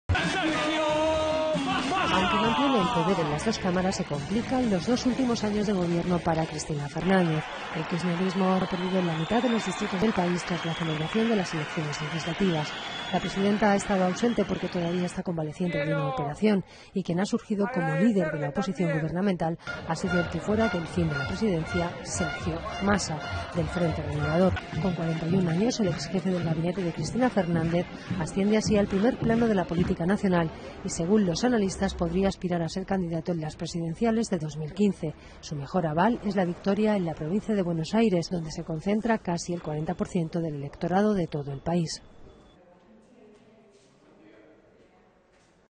Cristina Fernández ha perdido en la mitad de los distritos de Argentina
Aunque mantiene el poder en las dos cámaras, se complican los dos últimos años de gobierno a Cristina Fernández, presidenta de Argentina. El Kichnerismo ha perdido en la mitad de los distritos del país tras la celebración de las elecciones legislativas. La presidenta ha estado ausente porque todavía está convaleciente de una operación. Y quien ha surgido como líder de la oposición gubernamental ha sido el que fuera delfín de la presidenta, Sergio Massa, del Frente Renovador.